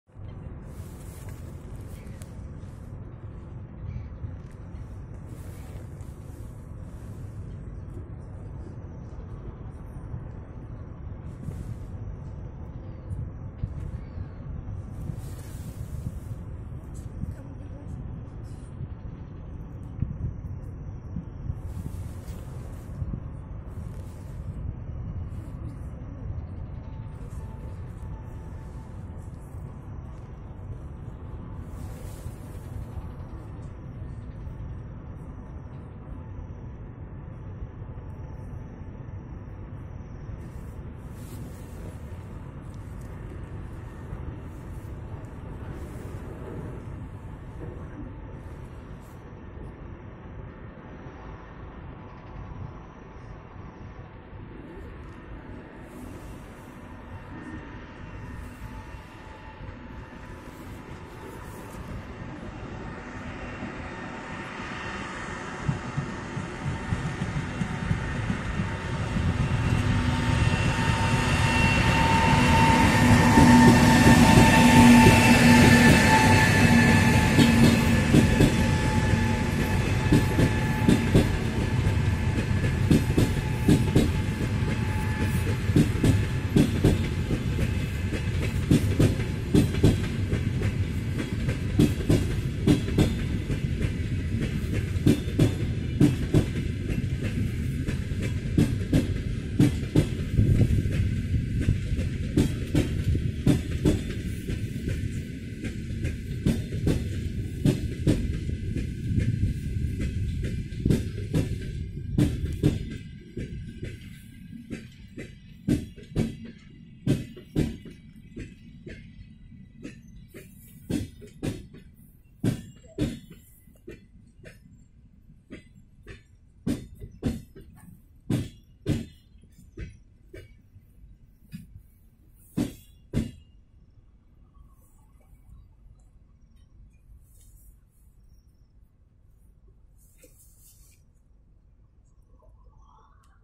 Электровоз ЭП20-031 с поездом 103 Адлер - Москва
Электровоз ЭП20-031 (ТЧЭ-6 Москва-Сорт. Рязанская) с поездом 103 Адлер - Москва заезжает на ст. Рязань-2, снято на ст. Рязань-2, 15:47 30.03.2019 г.

Снято на смартфон Xiaomi Redmi 5A с использованием штатива Xiaomi Bracket self-timer.